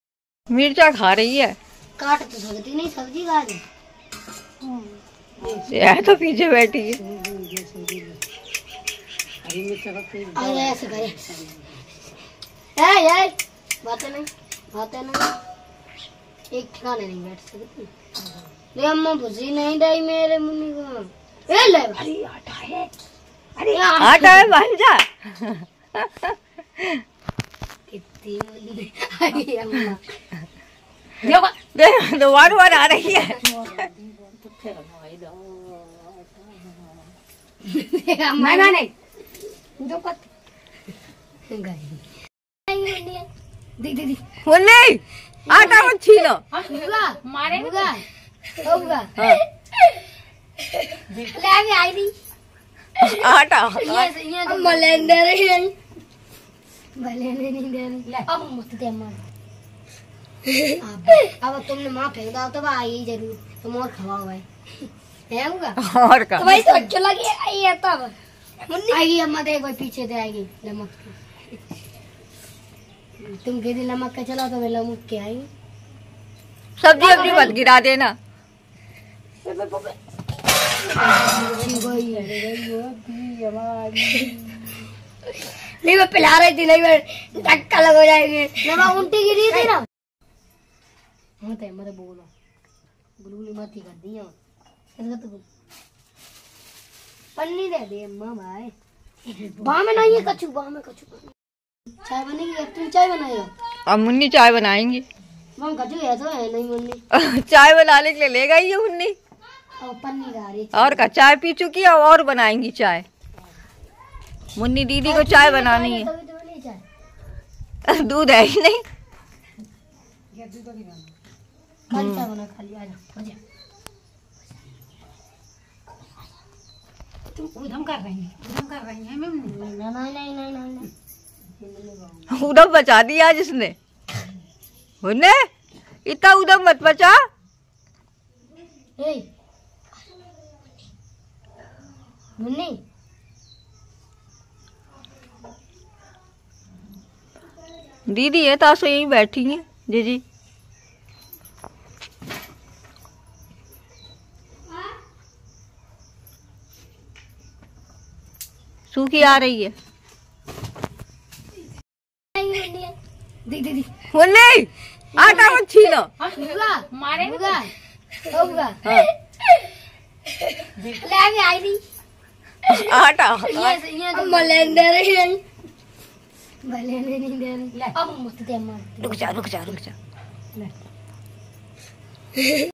मंकी मुन्नी और अम्मा(दादी) दोनों लोग खाना बना रहे है, monkey new funny video🐵🤣
मुन्नी आज अम्मा को (दादी) को खाना नही बनाने दे रही है,monke funny video